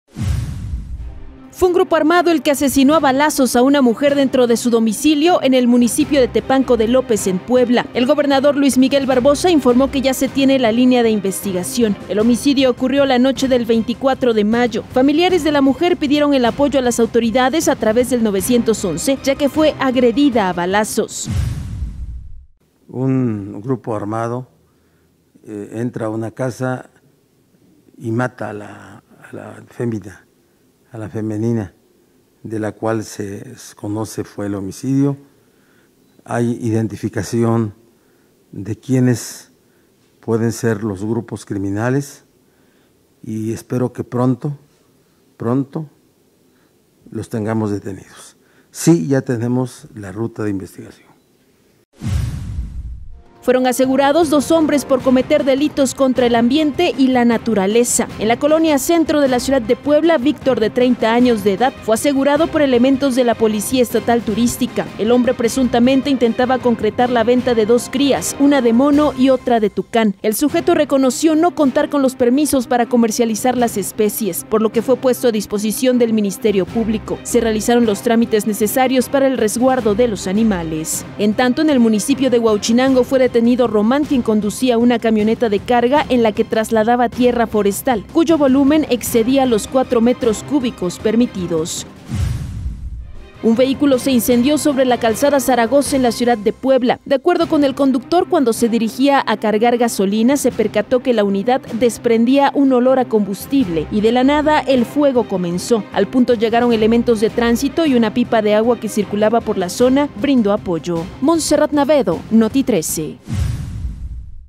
🔴 Hechos del día. Fue un grupo armado, el que ejecutó a mujer, en Tepanco.
🔴 Hechos del día... Fuer un grupo armado el que mató a una mujer en Tepanco de López; un hombre fue detenido al intentar vender a una cría de mono y otra de Tucán; un auto se incendió en Calzada Zaragoza.